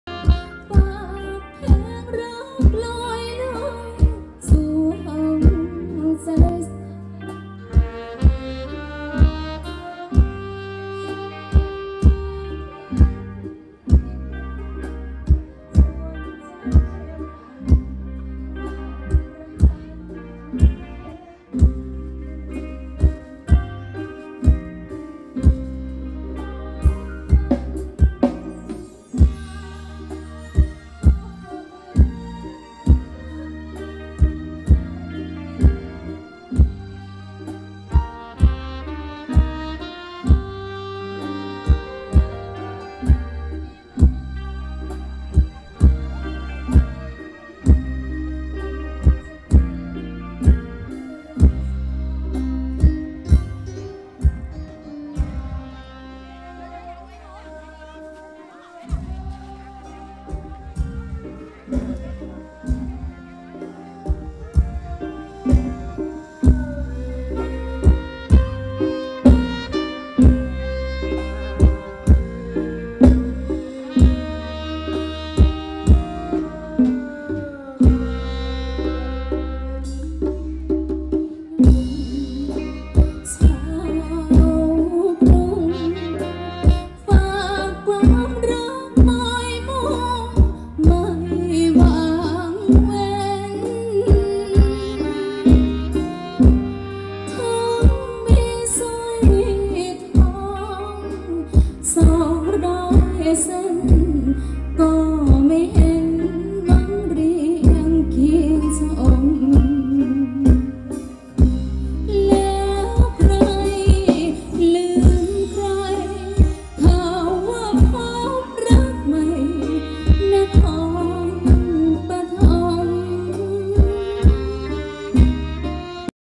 【PhetchaburiDance Workout for weight loss】รำวงเพชรบุรี✿Health&Beauty❤Entertainment♡Pretty♡
name of Dancer team : Nongbonapaporn,Phetsopa
name of Music Band : MangKoodPhet (mangM khootH Phet)
Show location : Ban Donklang,Photharam,Ratchaburi,Thailand
Date : August18,2018♡★